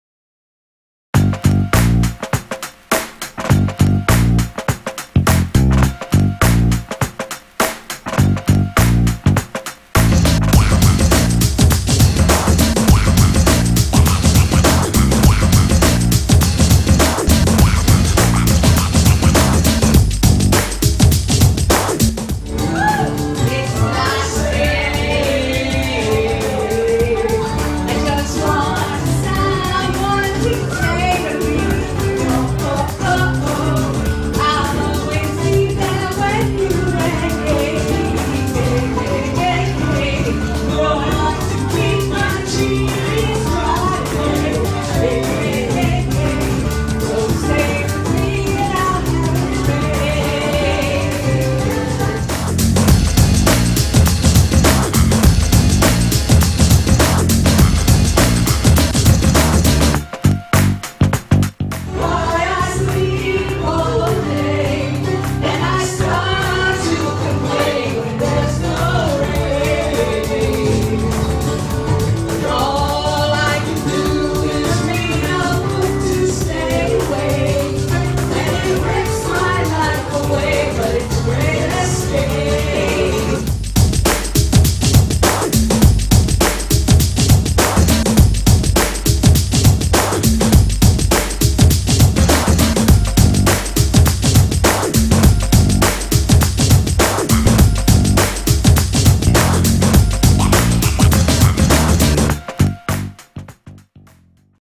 Meet The Weirds-Mary P. Performs
One of our fave Weirds, Mary P performs at Aqua Idol - raising funds for Switchboard 211 Helpline of the Florida Keys. We love our weirds.